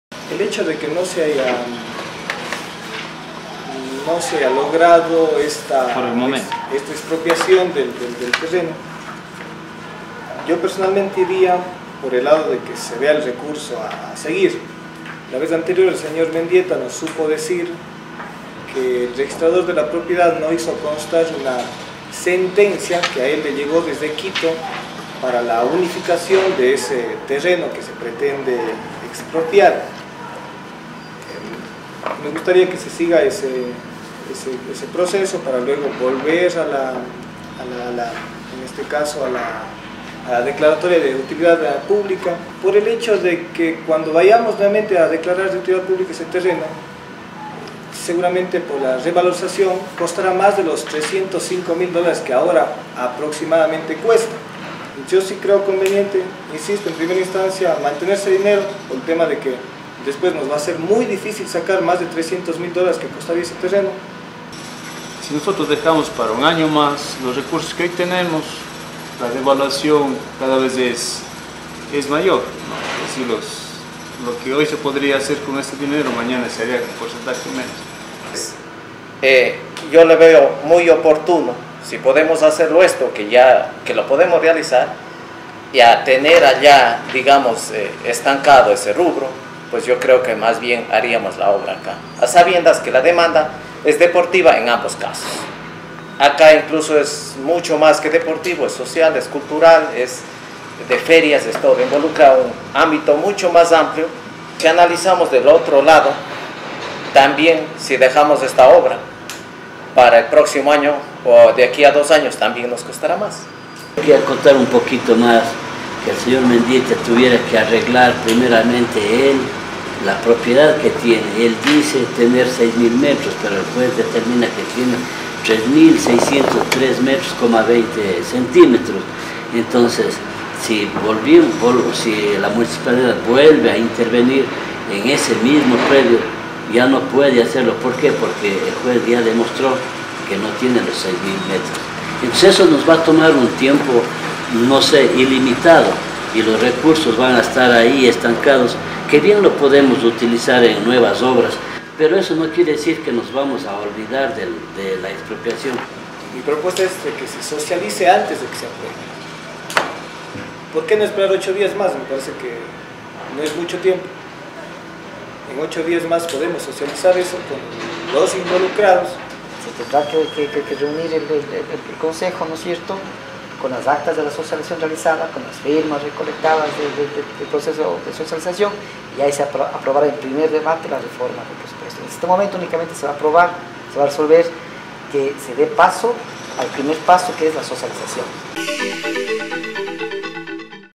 Recurso destinado a expropiación proponen invertir en techo de cancha
Para la expropiación del terreno junto al estadio de LDCG, el Concejo de Girón, en el presupuesto de obras de impacto cantonal del año 2013, destinó un monto de 150 mil dólares. Ante el dictamen del juez, alcalde y concejales de AP, plantearon la posibilidad de  invertir esos recursos en otras obras (techo en las canchas junto al Salón del Pueblo, cancha sintética en el complejo deportivo de Pambadel y estudios para una segunda planta del Centro Comercial Girón)
El concejal Fausto Ávila de PKTK por su parte propuso se conserve el recurso para un nuevo planteamiento de expropiación, o, en caso de no ser así, llevar los planteamientos a una socialización, misma que se cumplió el lunes 4 de agosto.